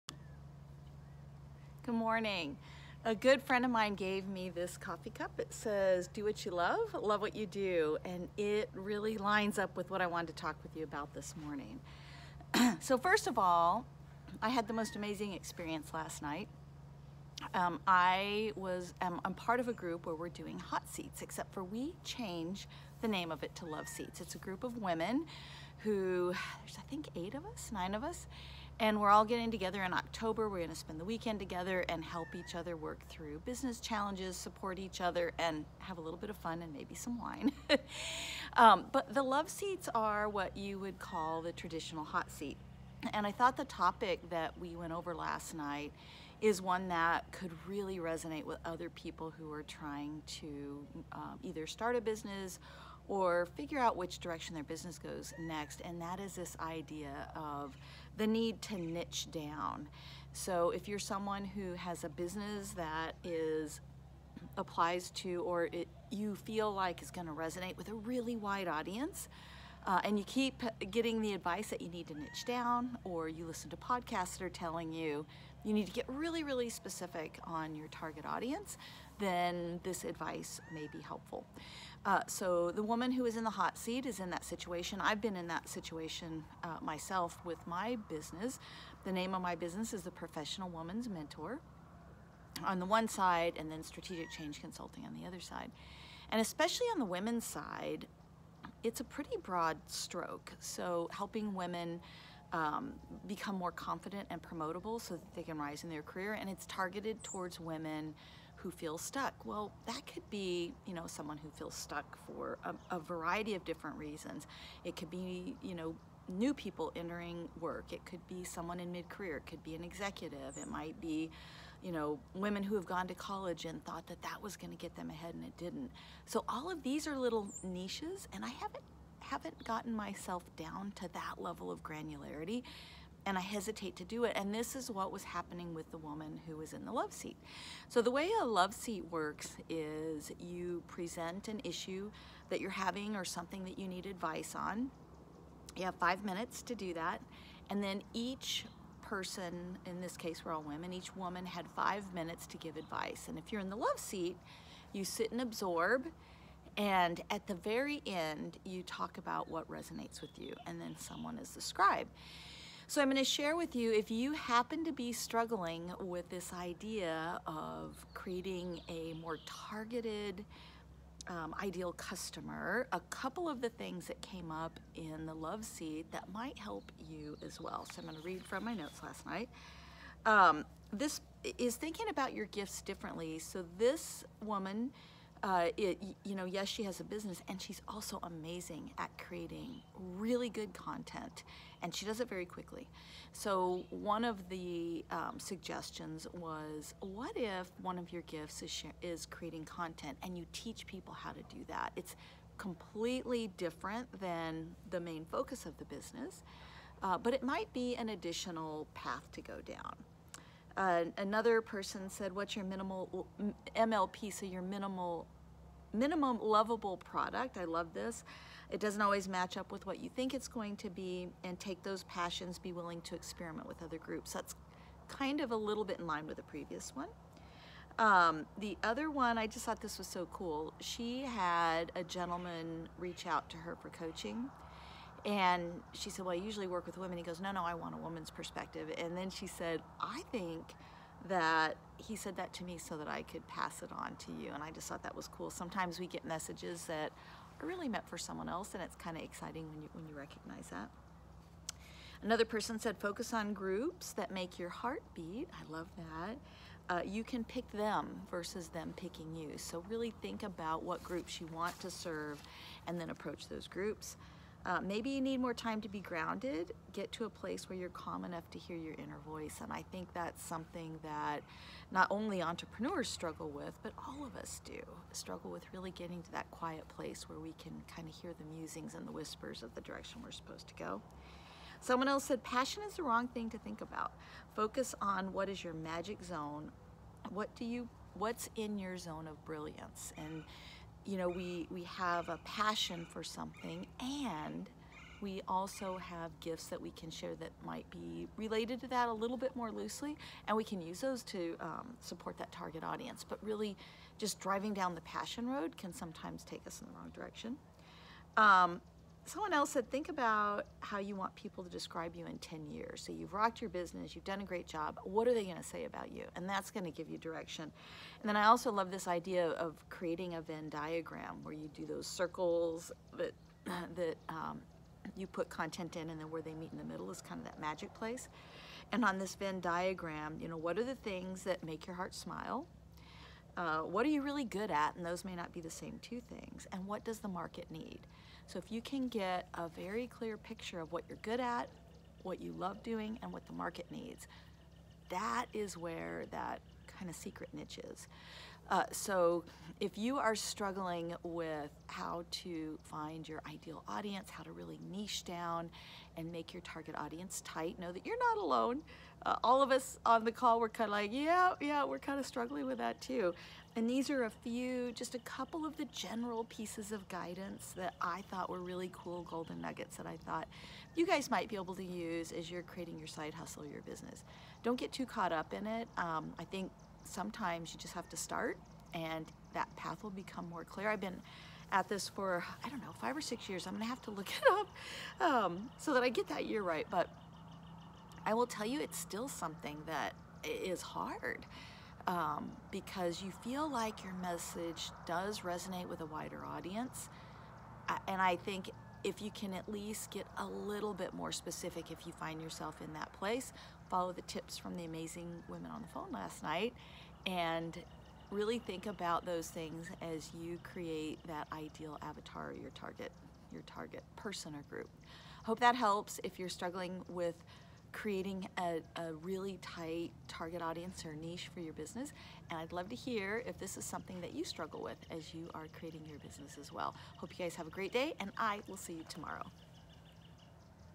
What's Your Niche? 💖 💫
An Entrepreneurs most dreaded question... What's Your Niche? If "defining your niche" is a struggle in your business, this video is for you!

Take a listen to learn the top tips from 9 amazing women on how to get clarity on who you serve, your gifts (they may be what you think they are), and the importance of listening to the whispers of your inner voice.

Do you have tip on how to better define your target audience? Let's learn together! 💖 💫